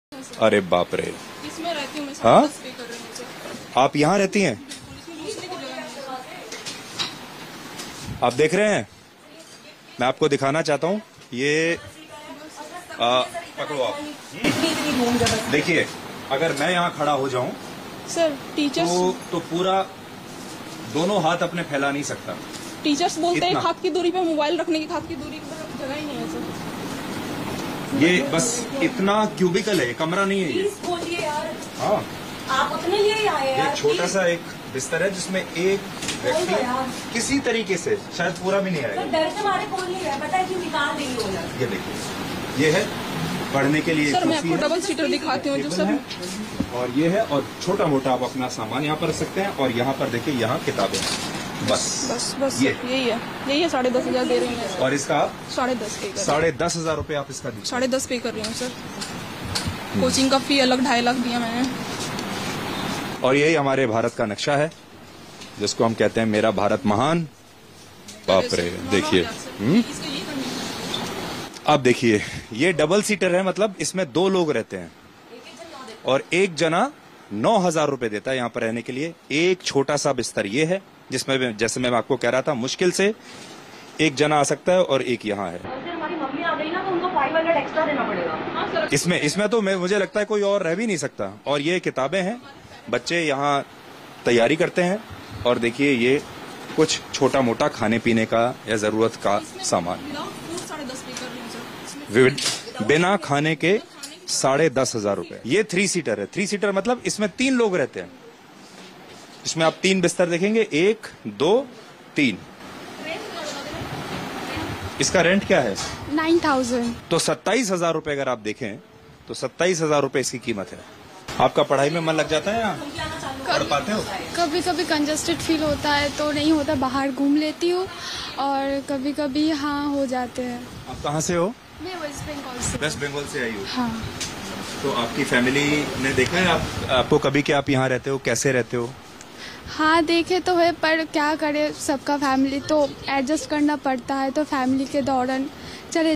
MCD Shuts Down Vikas Divyakirti's Institute: The Silence of Khan Sir & Ojha Sir Explained"
2. "Delhi Coaching Centre Sealed: Vikas Divyakirti's Troubles & The Quiet from Khan Sir & Ojha Sir"

"The Municipal Corporation of Delhi (MCD) has recently sealed Vikas Divyakirti's coaching centre, a major hub for UPSC aspirants. This unexpected action has left students in turmoil and raised questions about the legality and fairness of the decision. Surprisingly, notable educators like Khan Sir and Avadh Ojha have not spoken out about the incident. In this video, we investigate the reasons behind the MCD's crackdown, the repercussions for students, and why prominent figures in the coaching community have chosen to remain silent. Watch as we uncover the complexities of this issue and its impact on the education sector in Delhi."